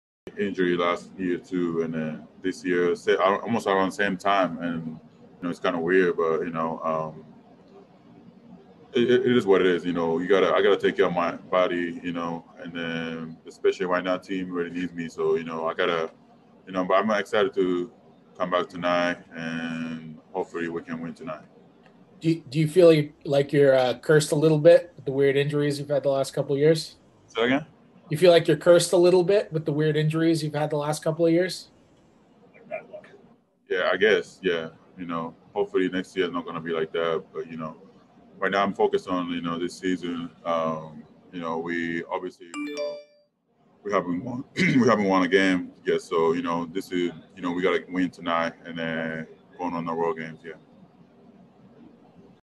Washington Wizards Presser: Rui Hachimura talks injury and return to the team (12-31-20)
Video Clip: Wizards PR